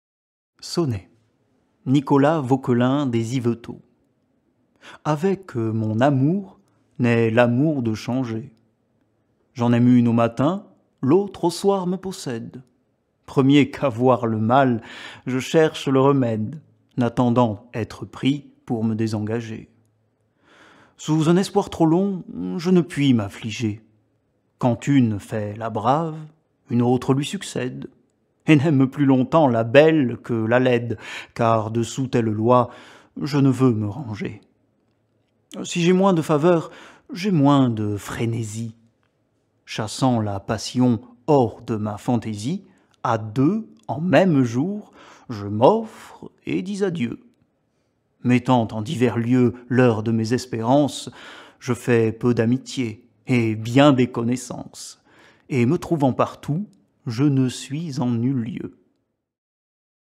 Vauquelin des Yveteaux (Nicolas) : SONNET - Avecques mon amour naît l'amour de changer.
Sonnet | Poème écrit par Nicolas VAUQUELIN DES YVETEAUX (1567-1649)
Extrait du recueil Œuvres complètes